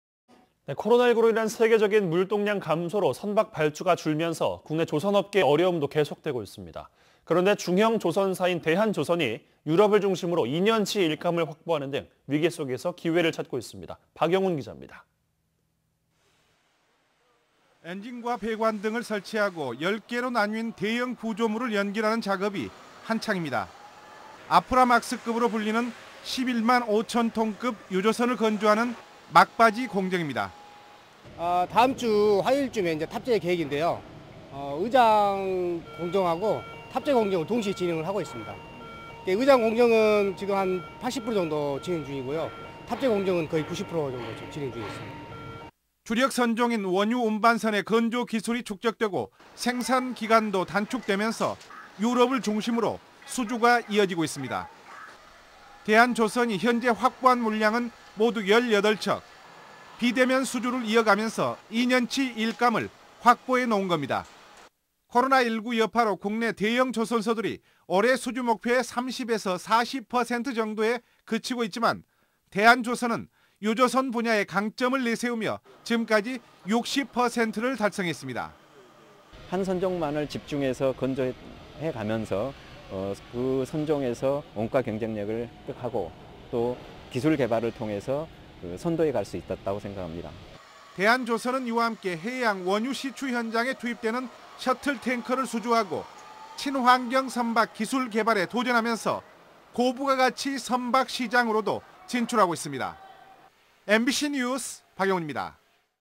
'유럽에서 빛나다'.. 위기에서 기회찾는 대한조선 - R (201019월/뉴스투데이)
◀ＡＮＣ▶
 코로나 19로 인한 세계적인 물동량 감소로
선박 발주가 줄면서 국내 조선업계의
어려움도 계속되고 있습니다.

 그런데 중형조선사인 대한조선이
유럽을 중심으로 2년치 일감을 확보하는 등
위기 속에서 기회를 찾고 있습니다.

 박영훈 기자가 취재했습니다.
  ◀ＥＮＤ▶

 엔진과 배관 등을 설치하고 10개로 나뉜
대형 구조물을 연결하는 작업이 한창입니다.

 '아프라막스급'으로 불리는 11만 5천톤급
유조선을 건조하는 막바지 공정입니다.

◀ＩＮＴ▶정춘석 직장 *대한조선 협력업체*
"다음 주 화요일 쯤 탑재 계획인데요. 의장 공정하고 탑재 공정을 동시에 진행하고 있습니다. 의장 공정은 지금 80% 진행중이고요.탑재 공정은 90%정도 진행중에 있습니다 "

 주력 선종인 원유운반선의 건조 기술이
축적되고 생산기간도 단축되면서 유럽을
중심으로 수주가 이어지고 있습니다.

 대한조선이 현재 확보한 물량은 모두 18척.

 비대면 수주를 이어가면서 2년치 일감을
확보해놓은 겁니다.

 코로나19 여파로 국내 대형조선소들이 올해
수주 목표의 30에서 40% 정도에 그치고 있지만
대한조선은 유조선 분야의 강점을 내세우며
지금까지 목표량의 60%를 달성했습니다.

 ◀ＩＮＴ▶신재범 부사장*대한조선*
"한 선종만을 집중해 건조해가면서 그 선종에서 원가경쟁력을 얻고 기술개발을 통해 선도해 갈 수 있었다고 생각합니다"

 대한조선은 이와 함께 해양 원유 시추 현장에
투입되는 셔틀탱커를 수주하고,
친환경선박 기술개발에 도전하면서 고부가가치 선박 시장으로도 진출하고 있습니다.